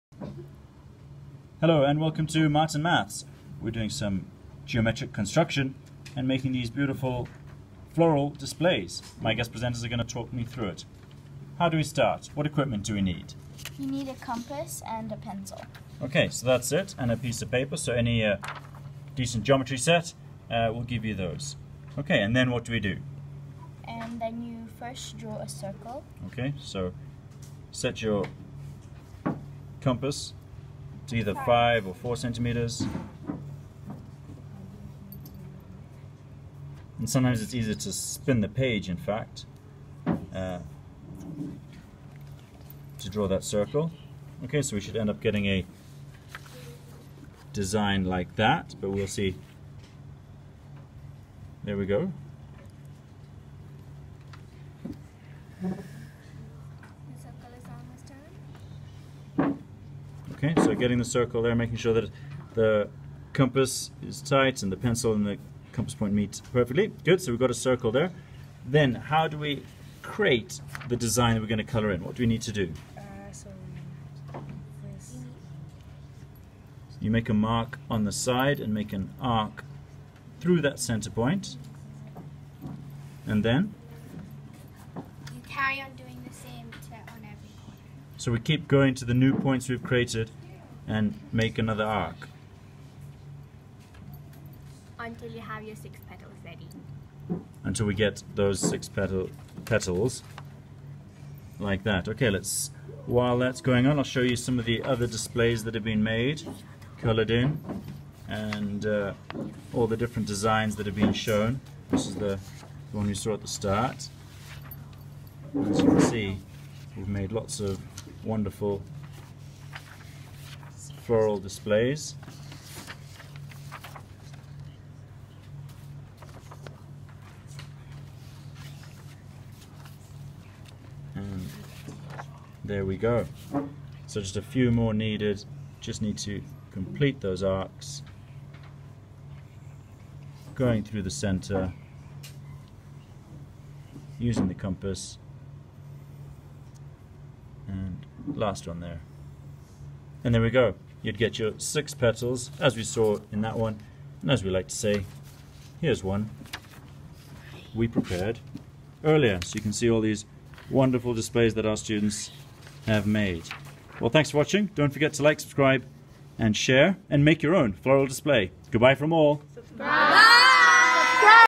Floral designs in circles
Construction of circle patterns, displays symmetry, rotation, reflection
Compass and sharp pencil needed!

can create hexagon by connecting points of flowers
Like petals of a flower!
also can create equilateral triangle, rhombus, trapezium, parallelogram, hexagon using the same skills

and flags!